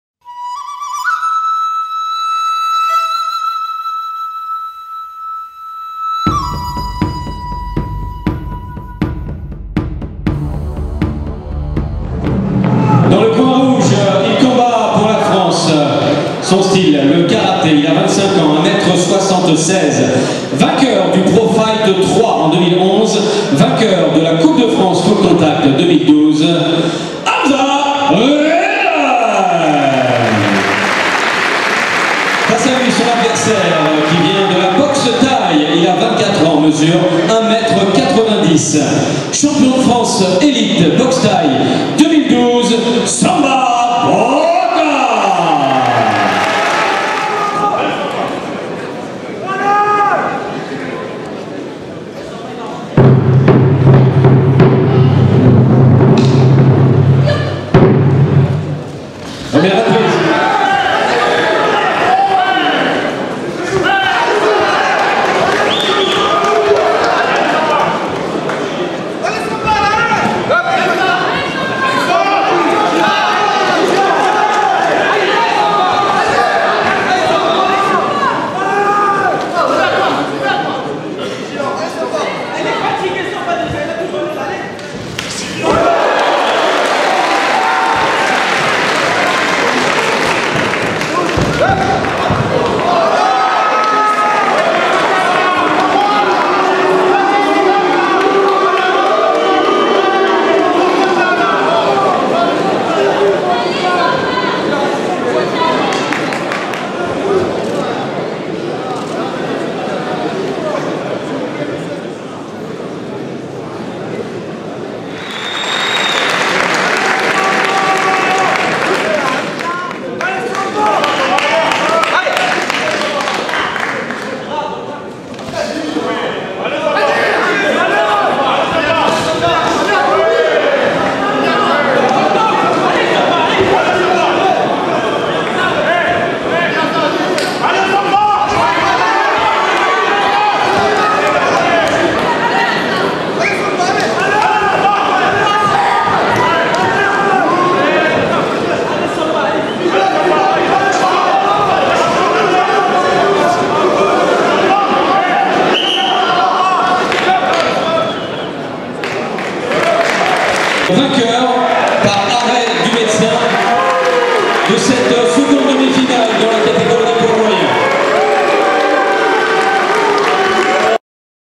Bocar vs Ridene - Demi-finale des moyens - PFK-4
Pro Fight Karate - 4
Demi-finale des moyens entre Samba Bocar et Hamza Ridène
Salle Marcel Cerdan - Levallois Perret - 30 juin 2012